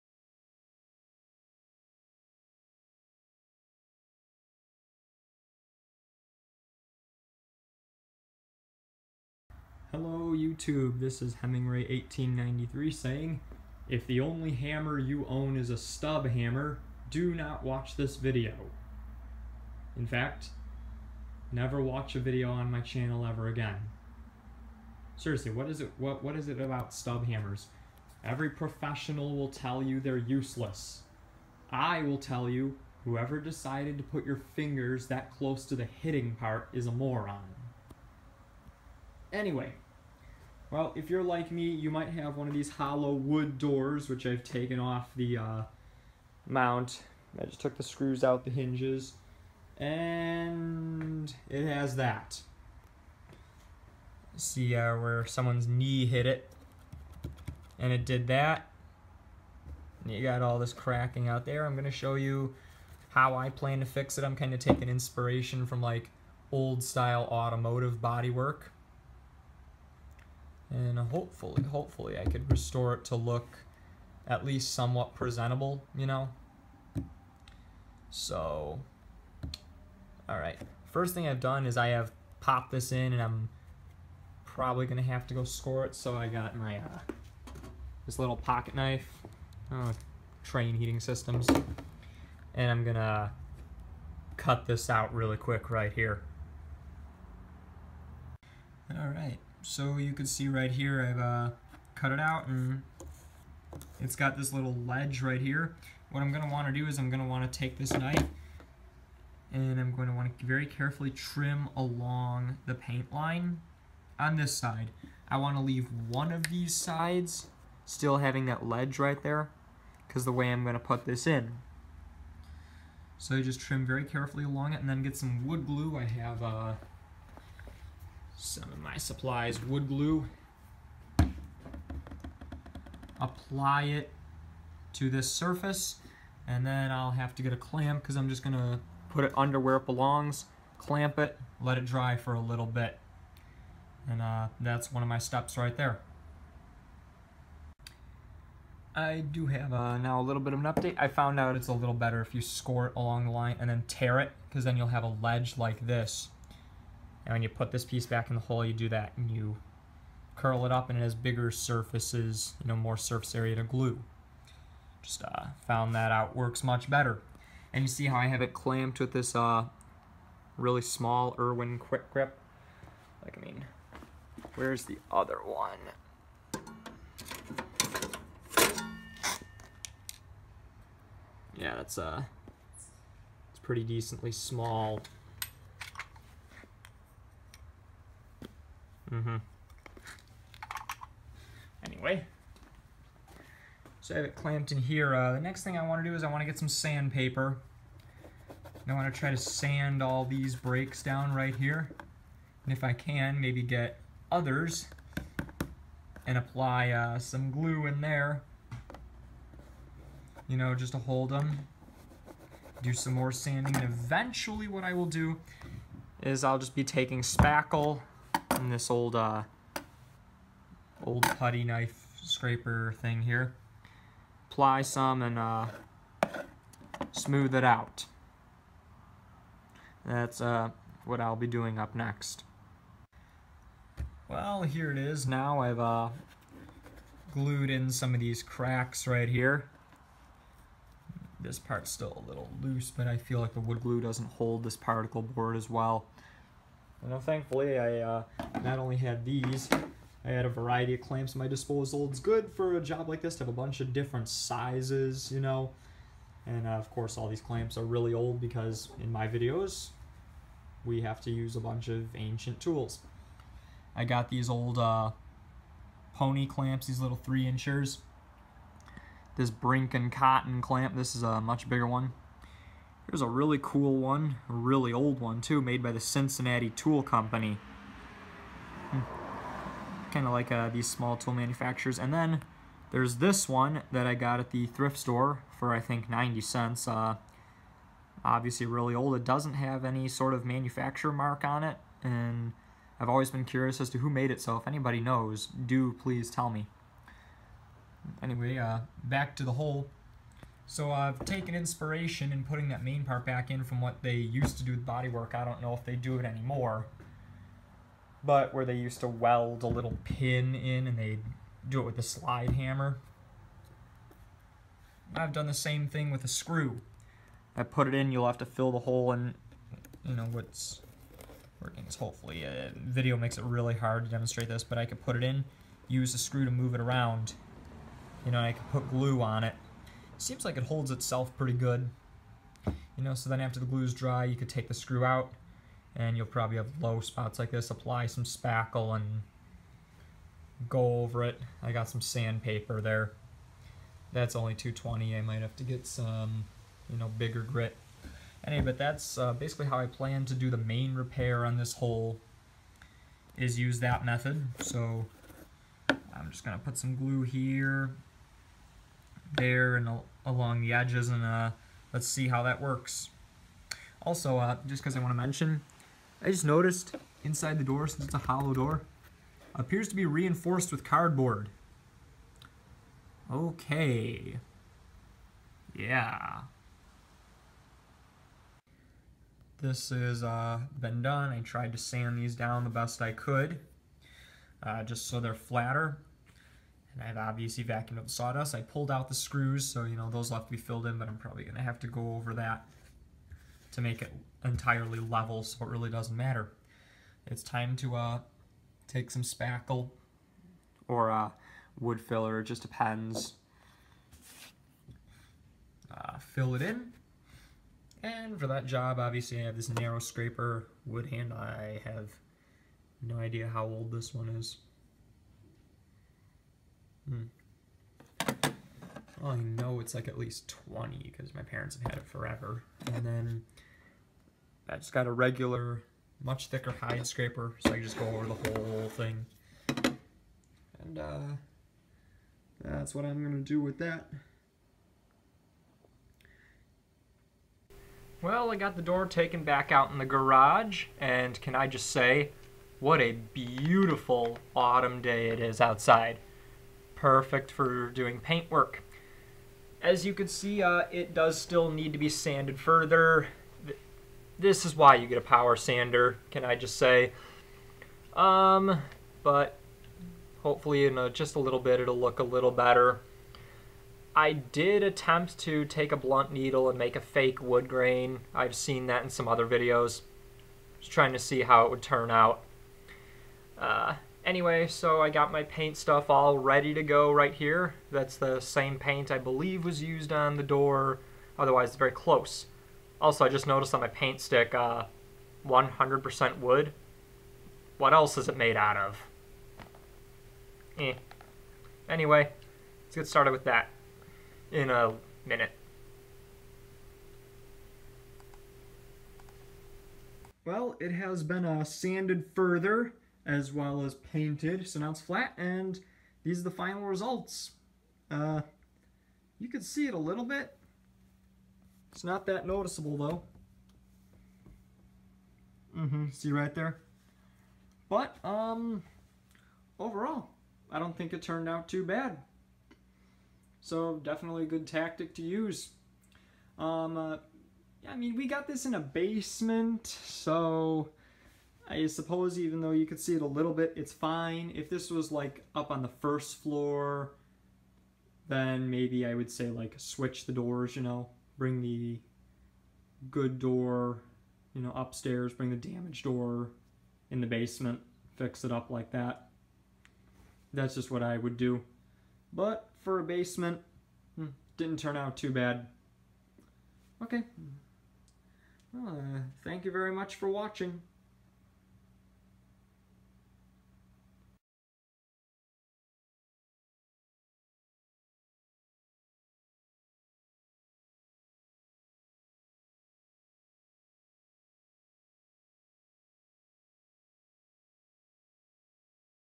Hemingray's Workshop: How to Repair a Hollow Door (Edit)
Hollow core doors break pretty easily, as many of us unfortunately find out. Most professionals will tell you to replace the door. But now, the homeowner can at least attempt to prove them wrong. As with any hollow door repair video, the results are not perfect, but turn out decent enough. Be very wary of any videos that claim the results to be flawless and completely unnoticeable.
         Also, as a side note, you don't actually have to take the door off the hinges. I did that to make it easier to work with.